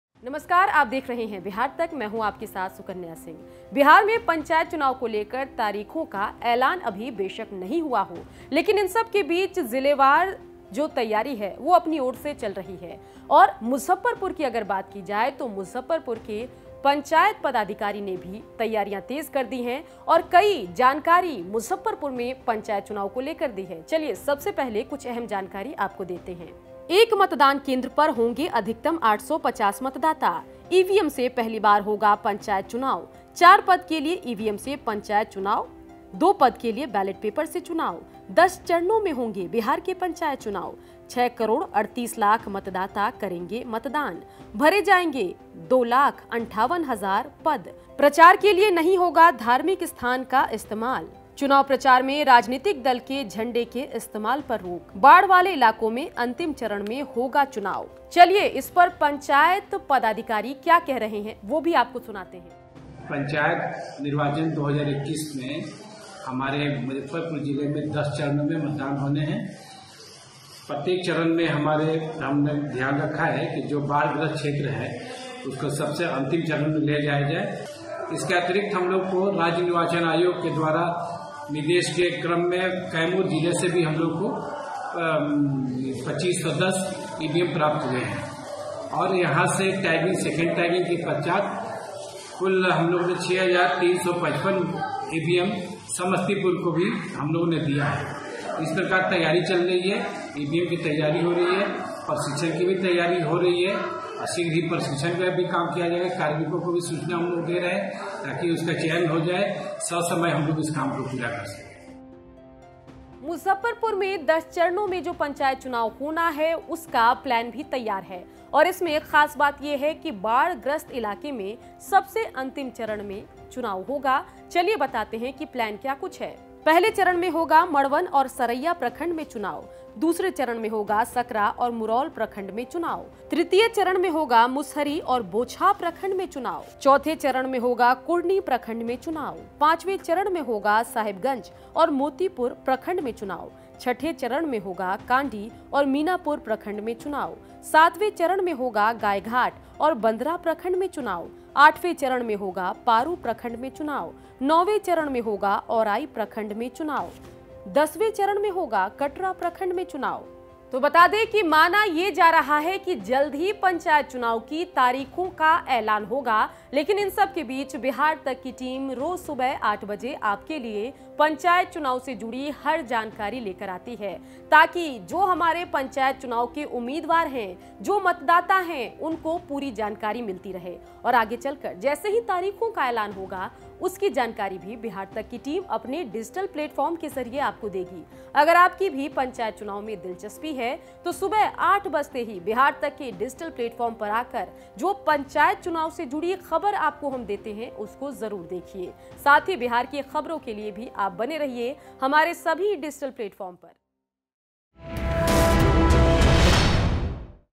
Bihar Panchayat Election : मुजफ्फरपुर का प्लान पंचायत चुनाव तैयार | Bihar Tak
Bihar Panchayat Election : मुजफ्फरपुर में जानिए कैसे कराई जाएगी पंचायत चुनाव में वोटिंग, क्या है जिला प्रशासन की तैयारी